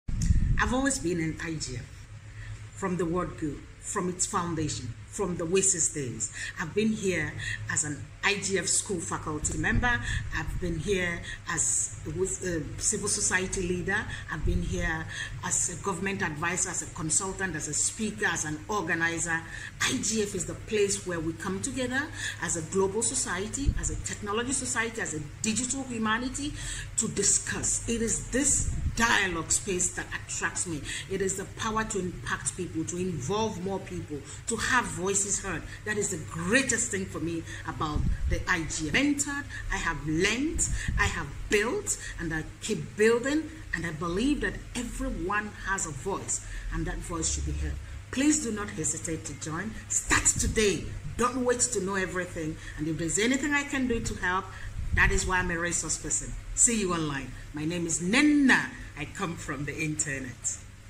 Nnenna Nwakanma - "Voices of the IGF"
Nnenna Nwakanma talks about her diverse roles at the Internet Governance Forum (IGF), where she has served as a faculty member, civil society leader, government advisor, consultant, speaker, and organizer. In this powerful testimonial, she highlights the IGF as a space for global dialogue, where digital communities unite to share ideas, mentor others, and amplify voices. Her message encourages everyone, especially newcomers, to get involved, share their perspectives, and help shape the future of digital governance.